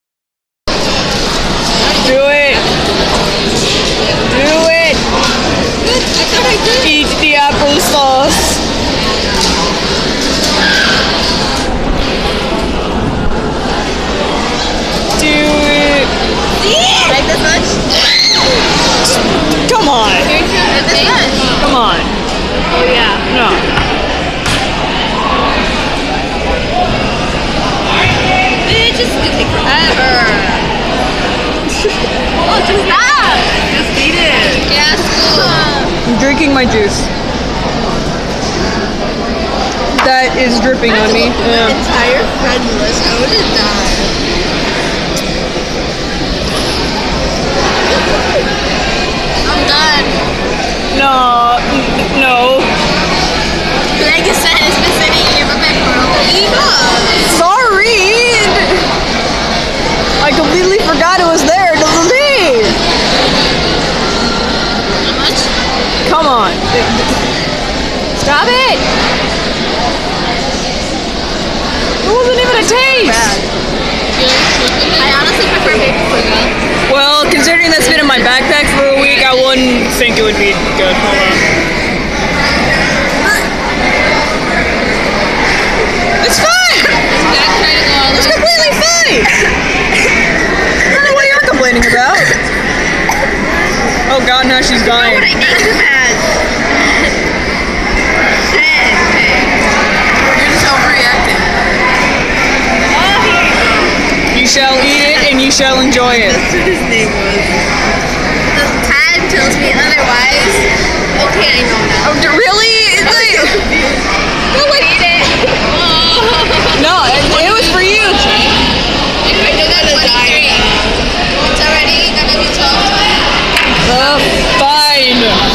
My Friend Tries Applesauce for the First Time
Yes, you read that correctly